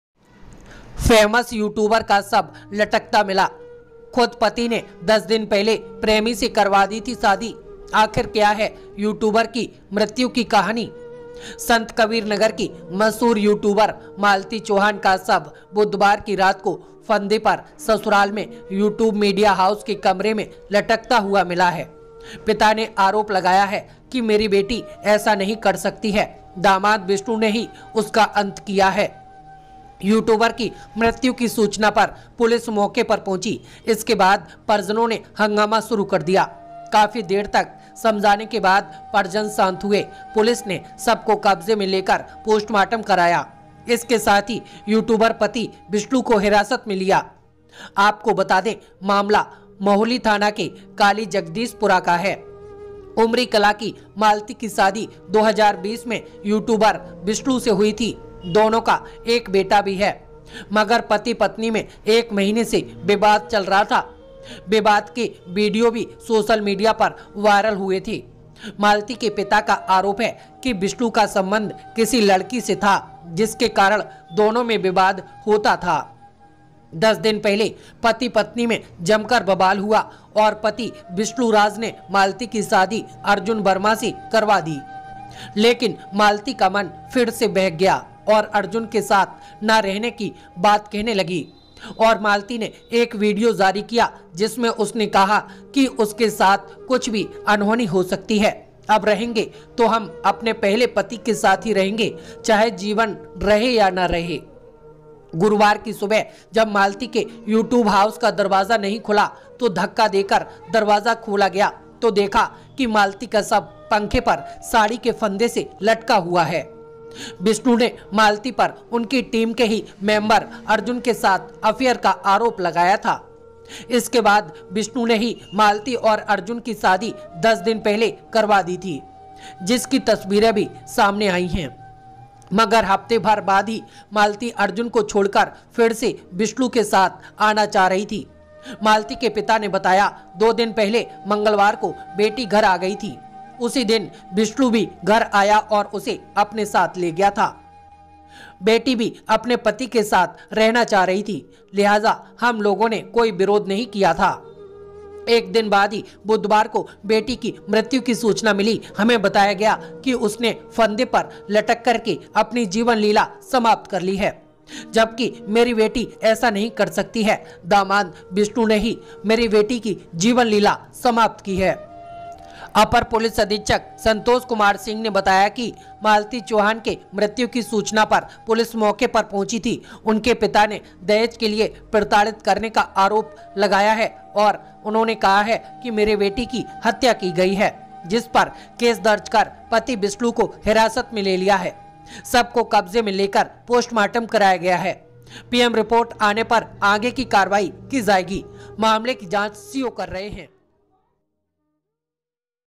पति ने फैमस Youtuber मालती चौहान (पत्नी) की खुद प्रेमी से करवाई शादी , मिला शव ..आखिर क्या है वजह..
Doot Samachar

पति ने फैमस Youtuber मालती चौहान (पत्नी) की खुद प्रेमी से करवाई शादी , मीडिया हाउस में मिला मालती का शव ... आखिर क्या है वजह...